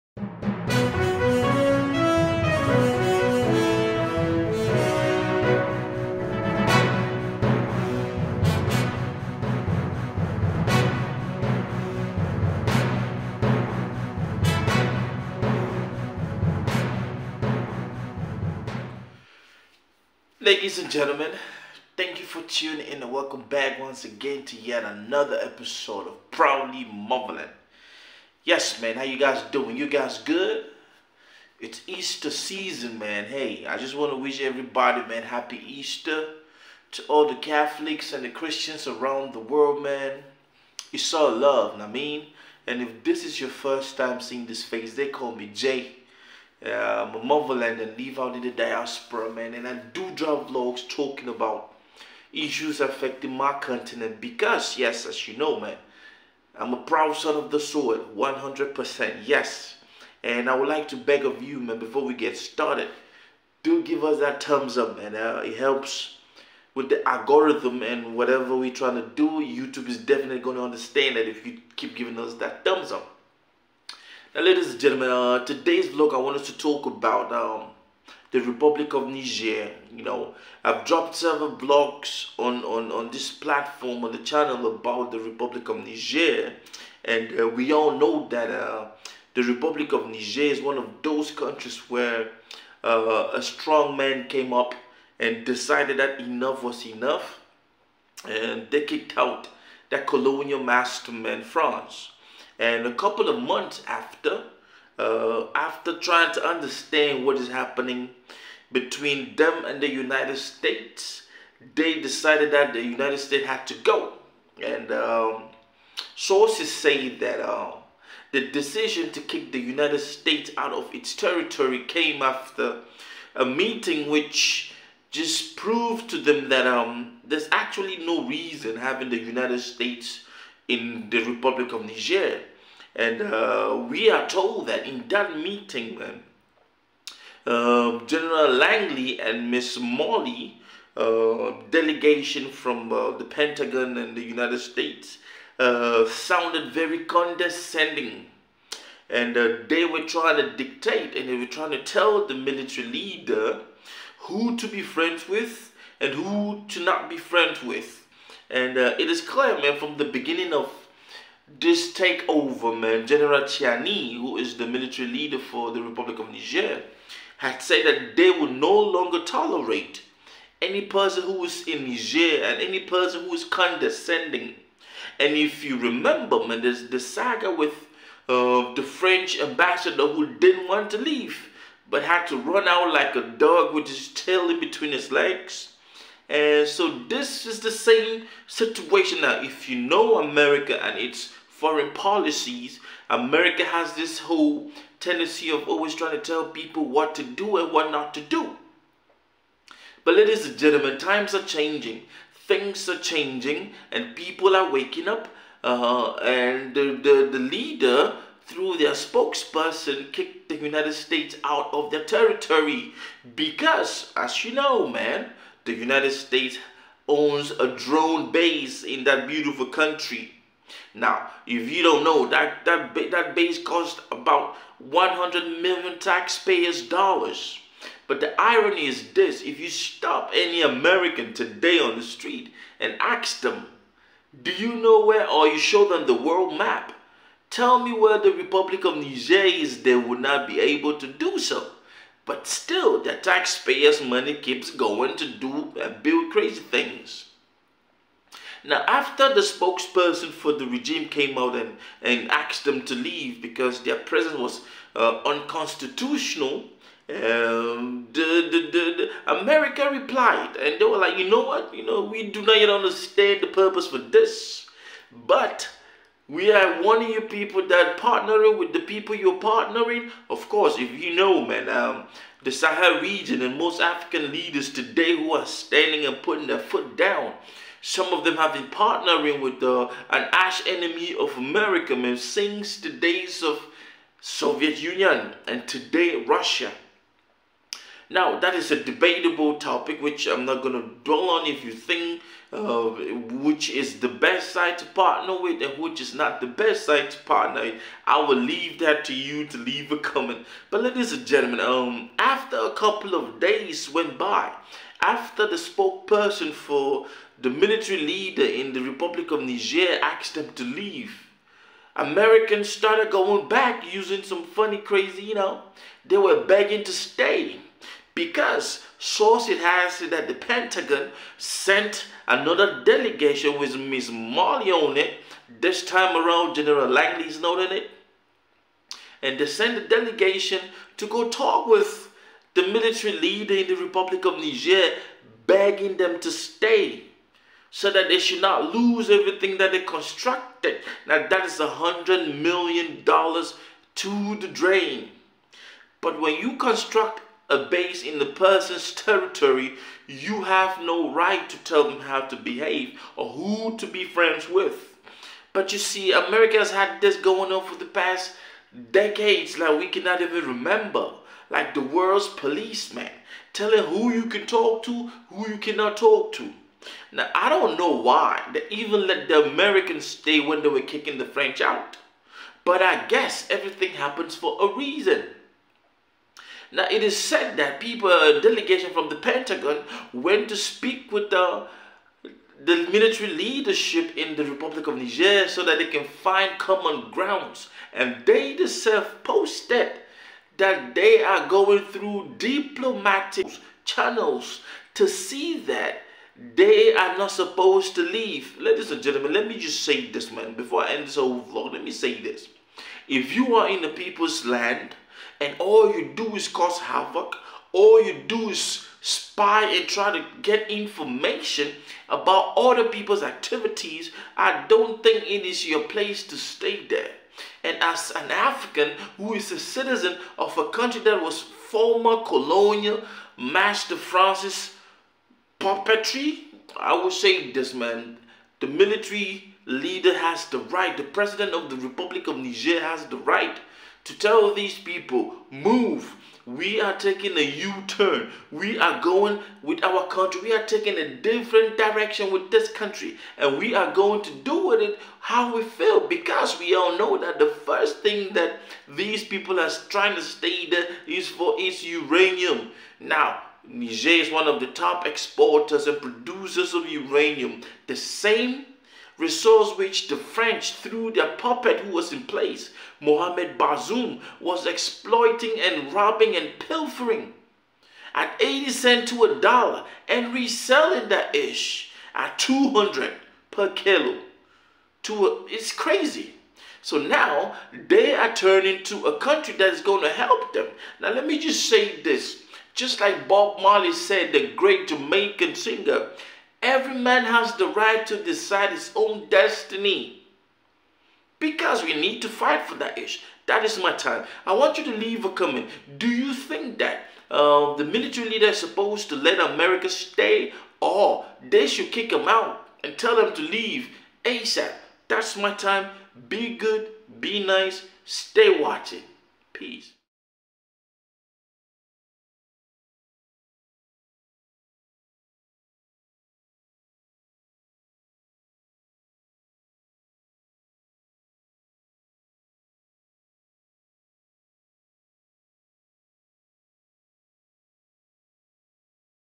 You won't believe it, America Begged Niger to Stay
After kicking France out of its territory, The republic of Niger kicked America out. However, it seems as if the Americans don't wanna leave and are starting to realize that the tables are turning, so the rush back and beg to stay and ask the Nigerien government to rethink.